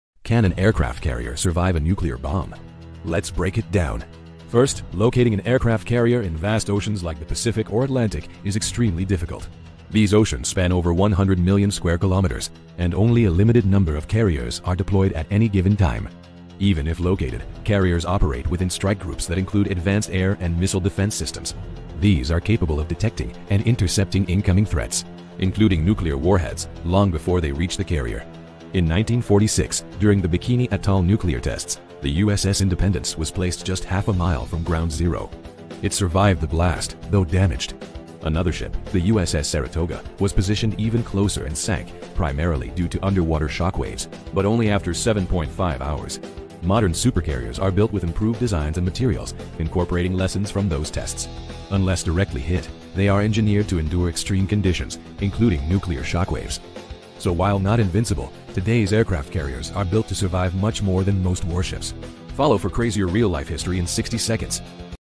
What Happens If a Nuke Hits an Aircraft Carrier?
How tough are U.S. aircraft carriers?
In this short, we break down whether a nuclear bomb could take out one of the most advanced warships on Earth. From real nuclear test data to modern defense systems, here’s what the evidence says.

Watch till the end to learn how today's carriers are designed to survive even the most extreme attacks.